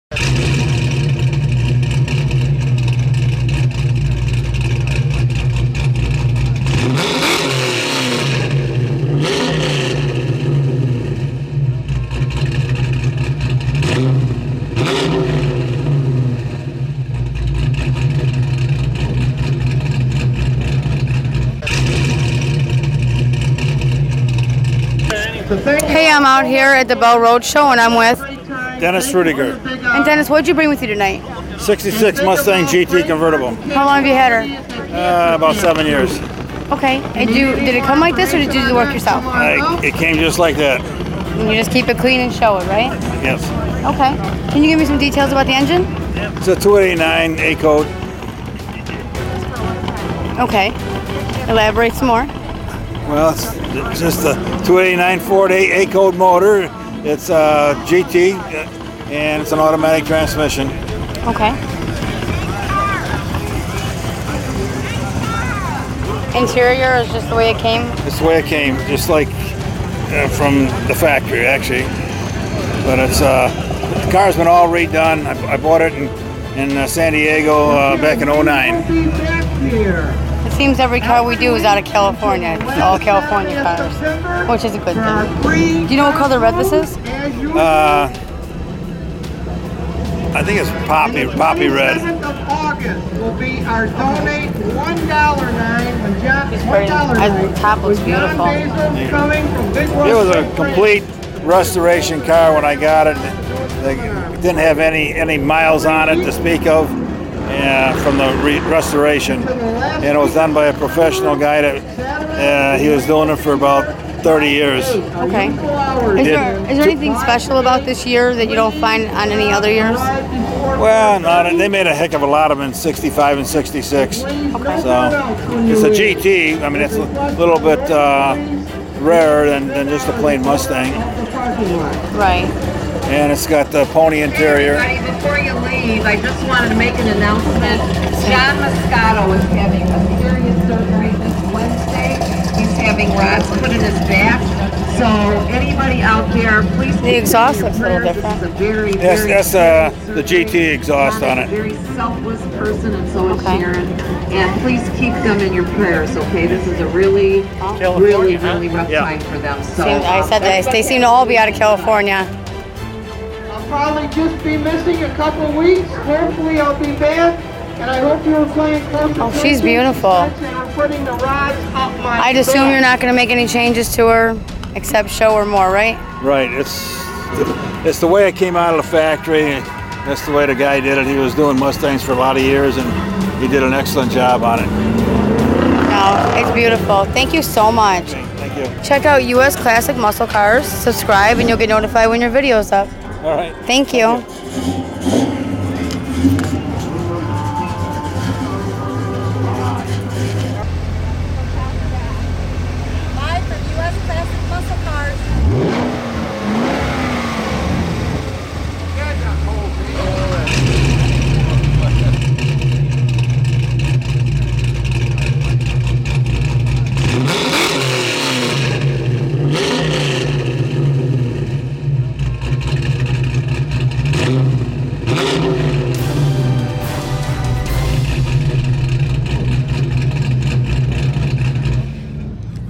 1966 Ford Mustang GT Convertible - Homer Glen Car Show (4K)
A 1966 Ford Mustang GT  with it being a convertible and great choice of colors - black on red